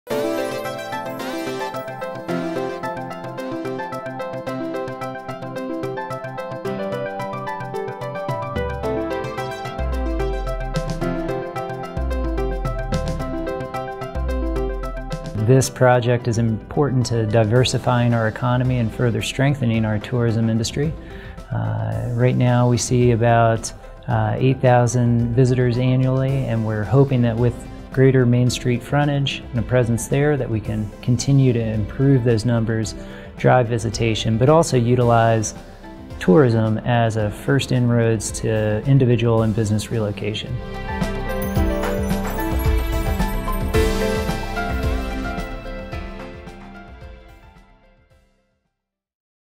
Lander to build a Chamber of Commerce and Visitor Center
Lander received a $466,210 grant and $194,316 loan to build a 3,400 square-foot Chamber of Commerce and Visitor Center. The project is intended to attract, enhance and increase visitation to the region, provide handicap-accessible facilities, offer extra office space and retain and expand businesses by providing services as a chamber and business complex.

Filmed 10/6/16